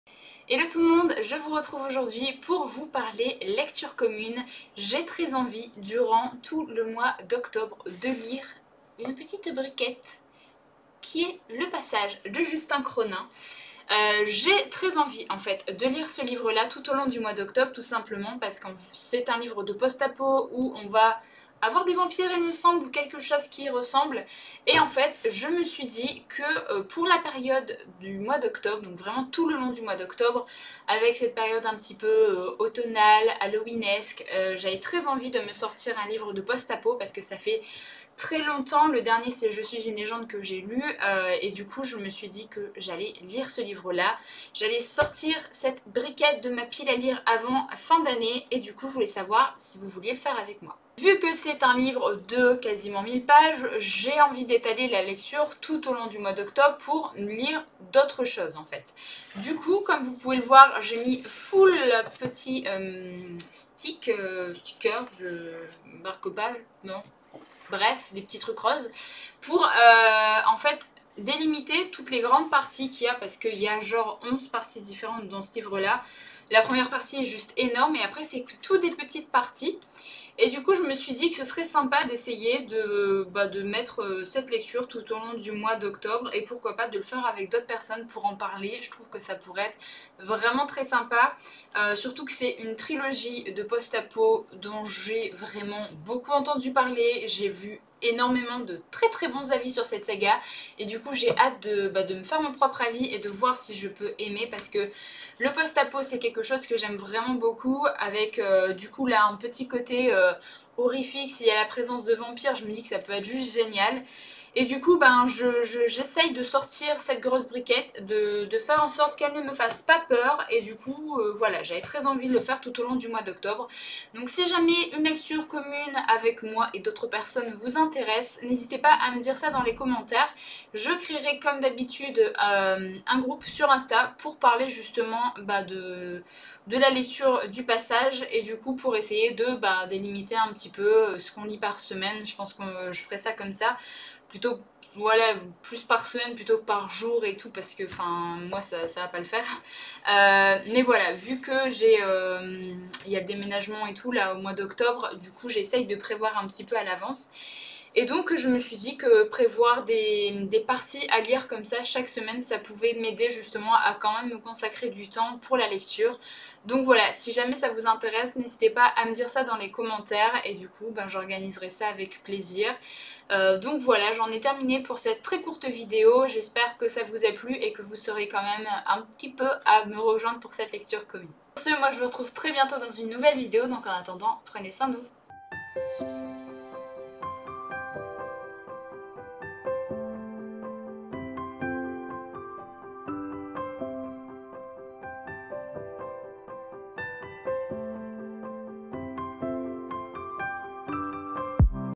On fait une Lecture commune ?
Hello!
Aujourd'hui je vous retrouve pour une courte vidéo ! Je vous propose de faire une lecture commune tout au long du mois d'octobre afin de lire la pavasse "Le passage" de Justin Cronin !

Si ça vous intéresse, n'hésitez pas à me laisser un commentaire pour me le dire! Je vais créer un groupe dédié à cette LC sur instagram ! :)

Livre cité:

Le passage

être tenu au courant de mes futures publications ! :)

Bonnes lectures !